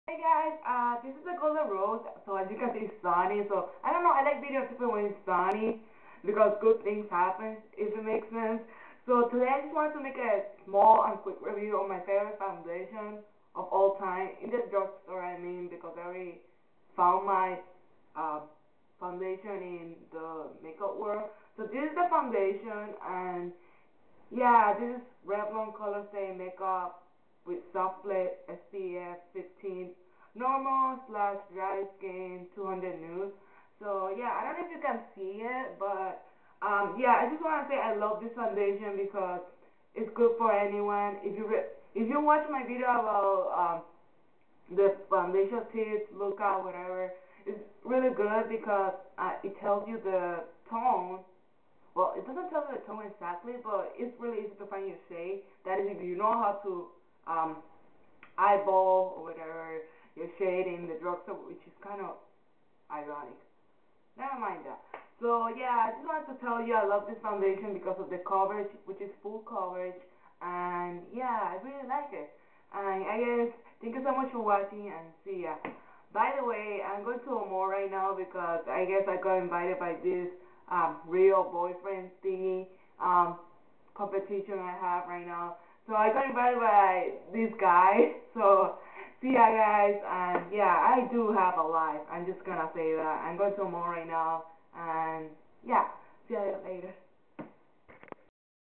Review: Revlon ColorStay Liquid Foundation
It is one of my favorite foundations in the Drugstore! Btw, this 100% honesty channel and I am just a teenager with a passion of makeup! ^^/ That is all people!

Featured Cosmetic Product...

Revlon ColoStay Liquid Foundation in Nude 200 for Dry/Normal Skin

Sincerely;
Amy or the "Golden Rose"